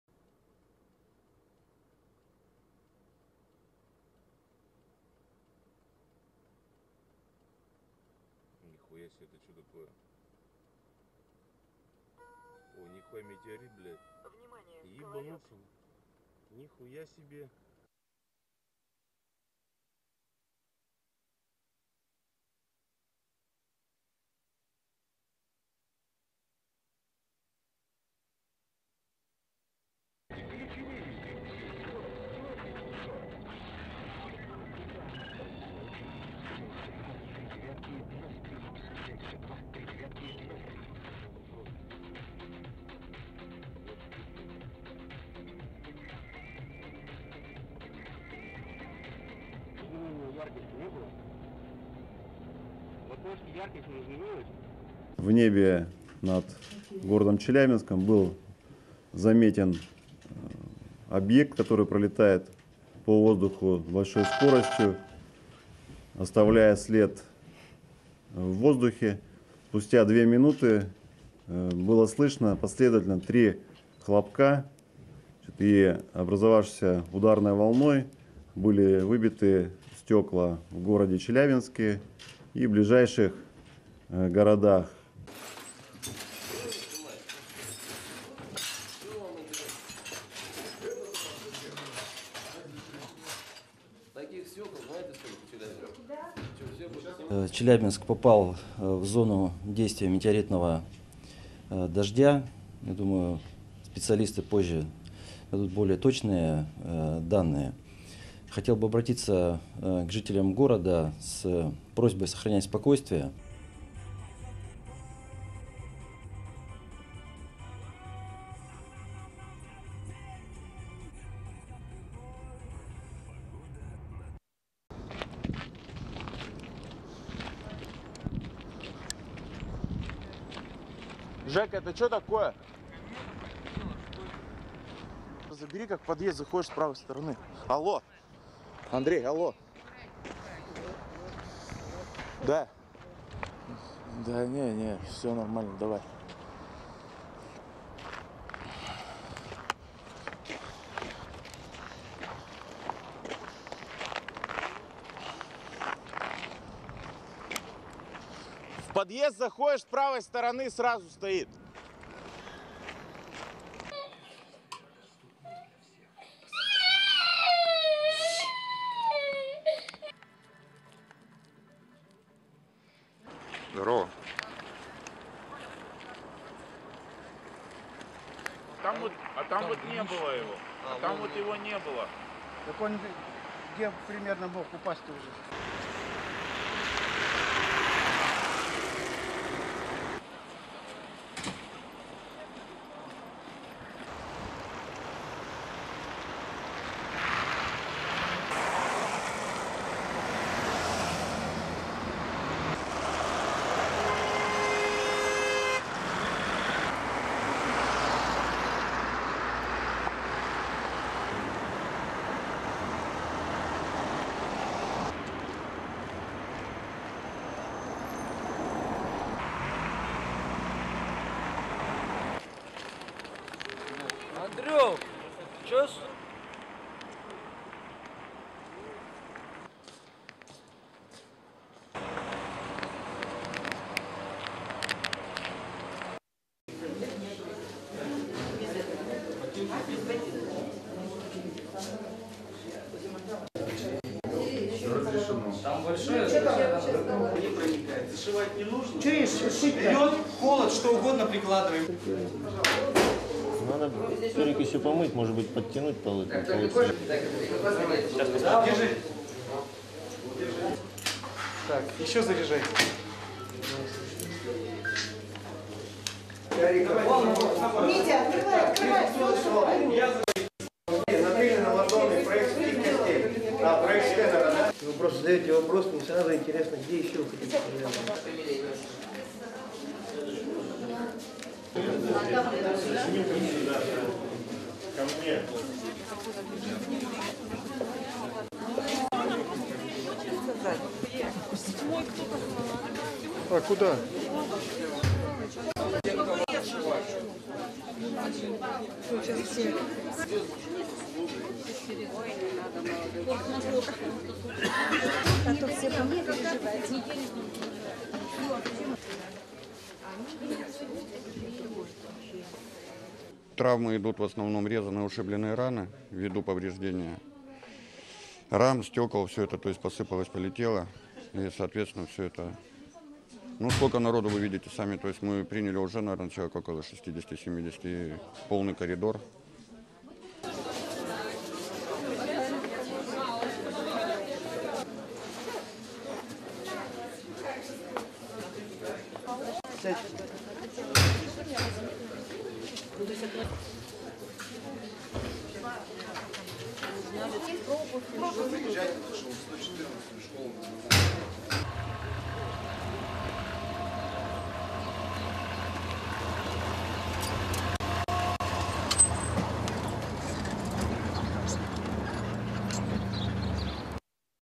Meteor Blows Out Windows and Injures Hundreds in Siberia
A meteor entered the atmosphere over Urals, Russia on February 15, 2013. The object created a sonic boom that shattered glass and injured hundreds of Russians. This video shows the meteor's entry, the aftermath and several people being treated for injuries at a local hospital.